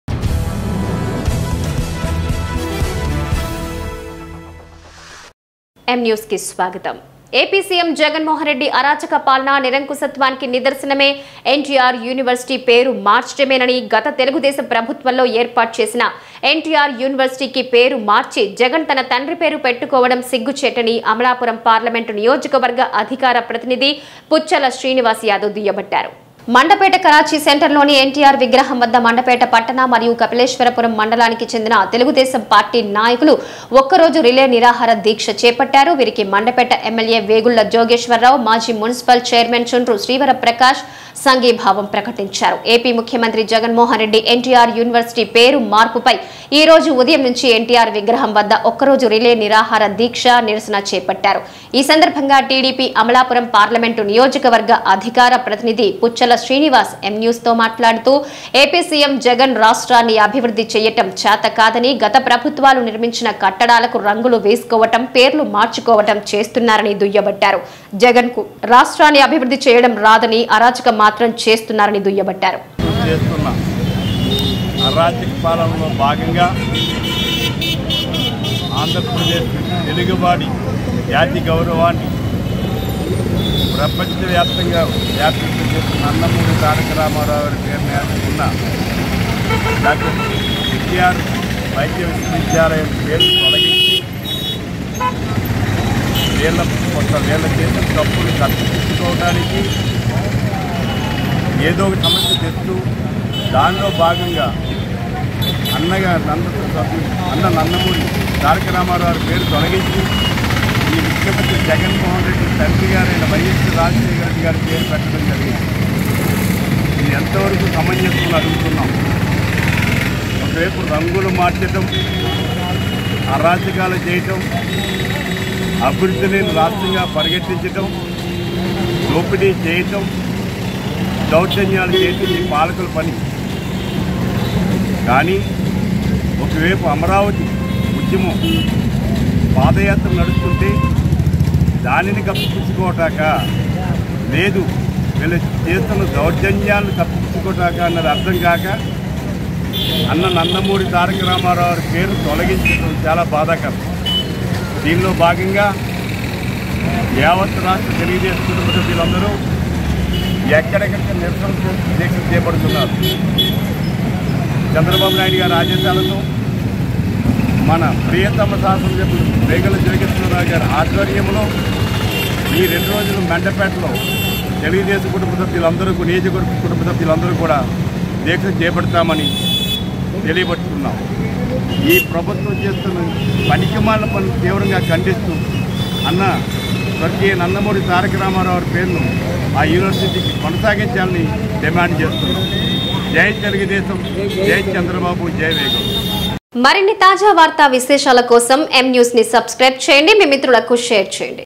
AP CM జగన్ మోహన్ రెడ్డి అరాచక పాలన నిరంకుశత్వానికి నిదర్శనమే- పుచ్చల శ్రీనివాస్ ||M NEWS TELUGU
AP CM  జగన్ మోహన్ రెడ్డి అరాచక పాలన నిరంకుశత్వానికి నిదర్శనమే NTR యూనివర్సిటీ పేరు మార్చటమేనని  గత తెలుగుదేశం ప్రభుత్వం లో ఏర్పాటు చేసిన NTR యూనివర్సిటీకి పేరు మార్చి జగన్ తన తండ్రి పేరు పెట్టుకోవడం సిగ్గుచేటని అమలాపురం పార్లమెంటు నియోజకవర్గ అధికార ప్రతినిధి పుచ్చల  శ్రీనివాస్  దుయ్యబట్టారు ..||M NEWS TELUGU